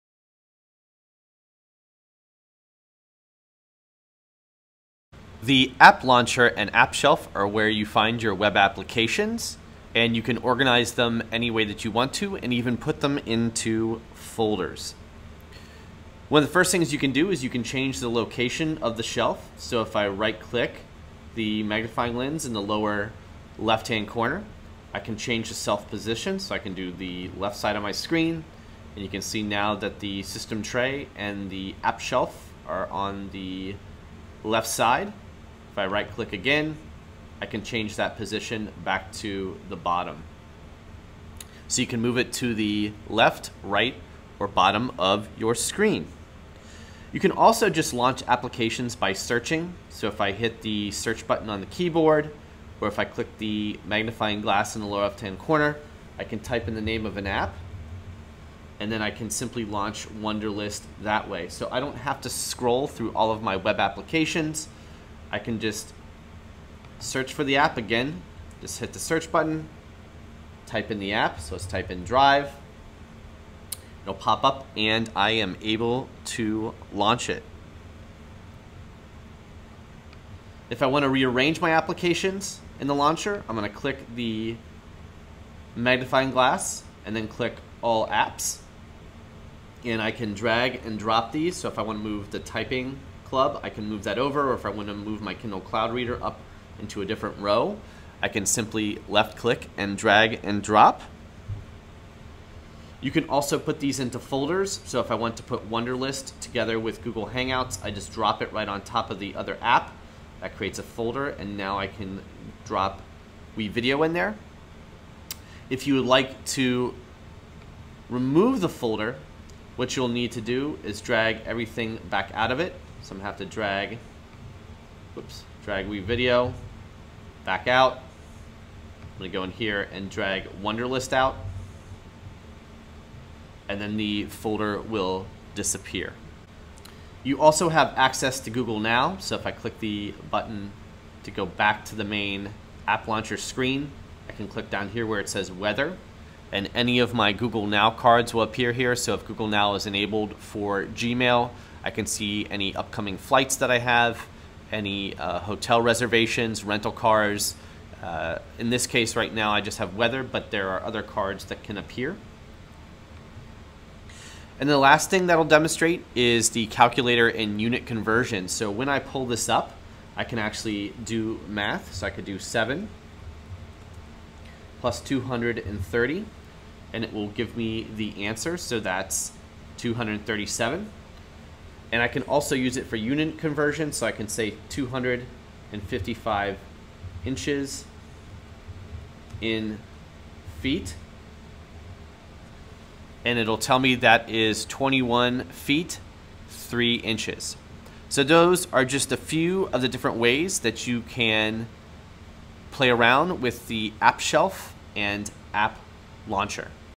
Chromebooks: App Launcher and App Shelf
Customize the order of your apps and even add folders to the app launcher.